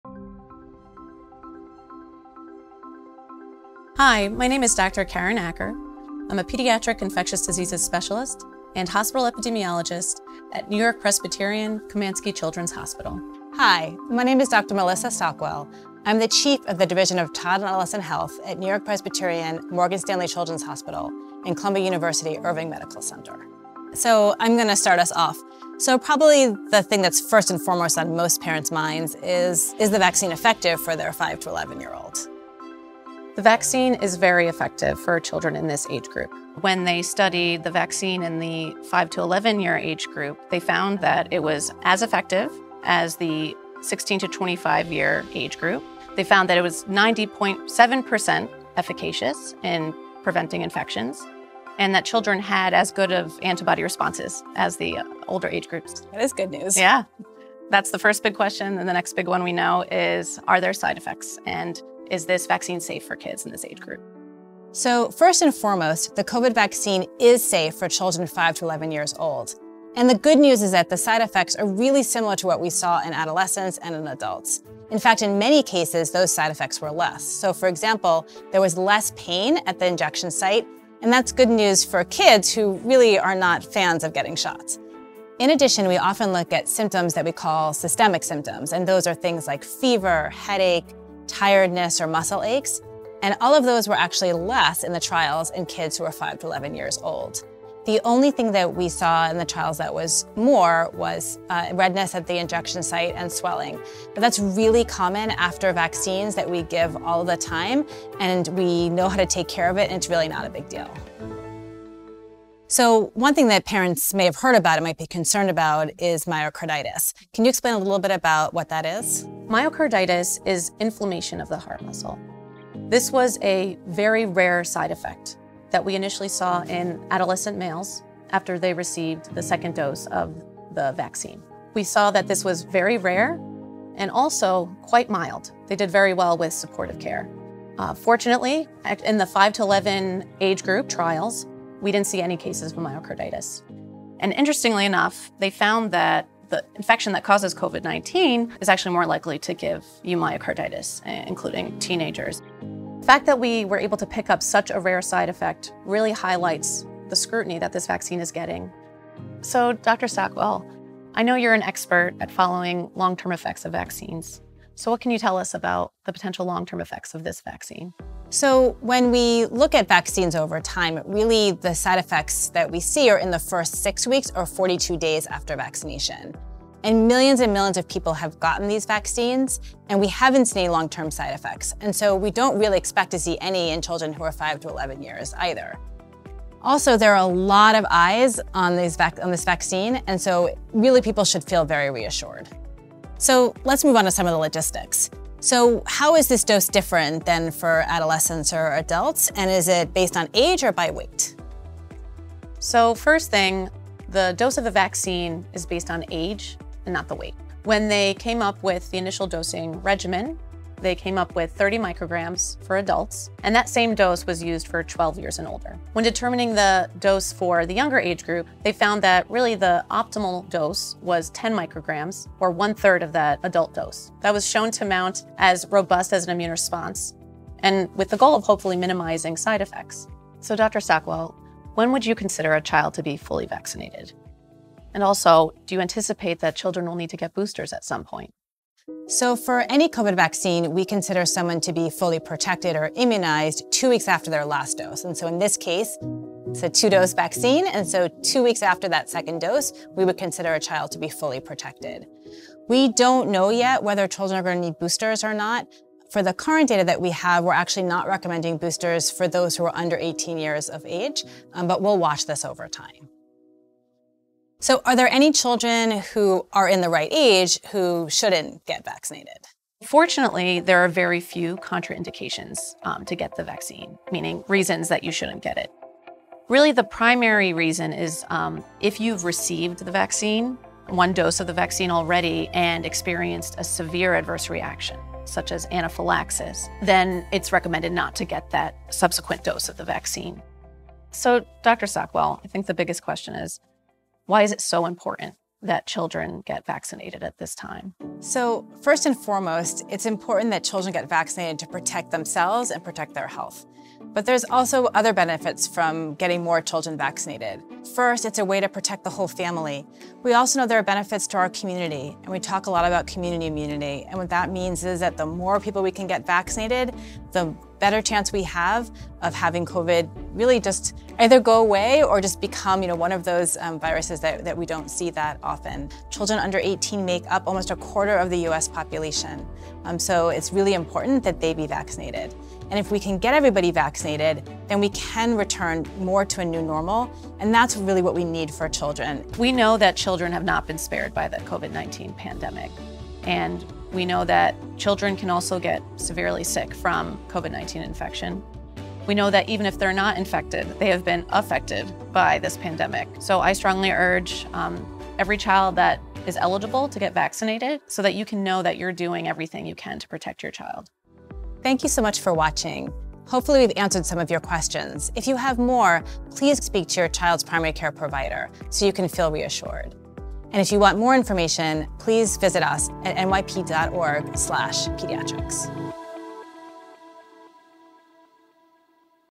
What to Know About the COVID-19 Vaccine and Young Kids
Two leading pediatric specialists answer parents’ questions about the COVID-19 vaccine for children ages 5 to 11.

Dr. Karen Acker, a pediatric infectious diseases specialist at NewYork-Presbyterian Komansky Children’s Hospital, and Dr. Melissa Stockwell, chief of the division of child and adolescent health at NewYork-Presbyterian/Columbia University Irving Medical Center, talk about the COVID-19 vaccine’s safety, side effects, and how effective it is at protecting kids ages 5 to 11.